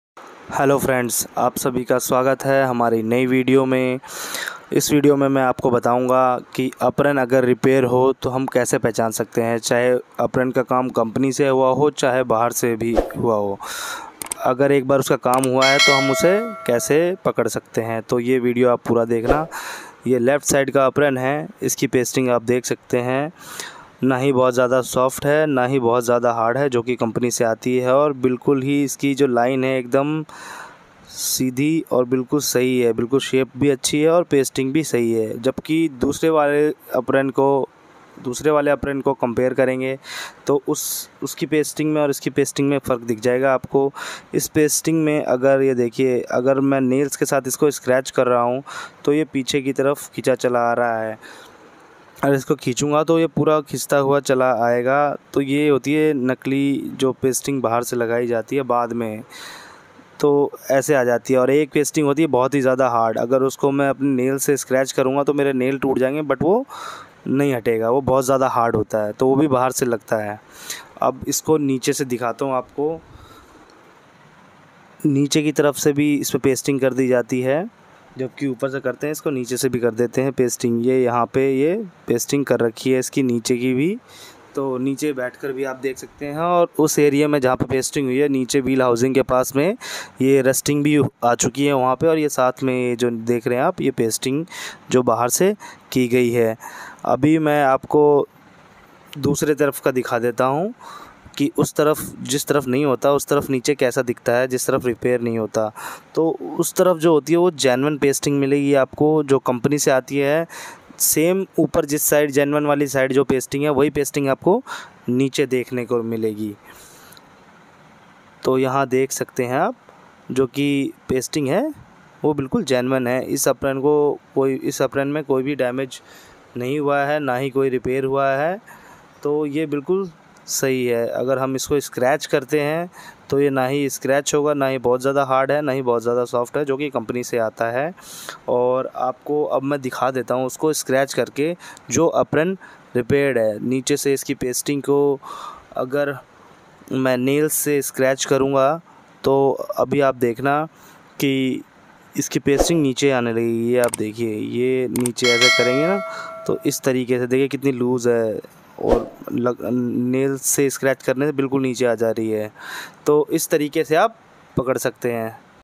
How to check Car Apron repair || कार का apron रिपेयर कैसे चेक करें
is video me mai apko btaunga ki car ka apron repair kaise check kar skte hai chahe repairing company se ho ya bahar se ham kaise repair check kar skte hai agr video pasand aye to video ko like ,share, subscribe, jarur kar dena apne friends ke sath share karna |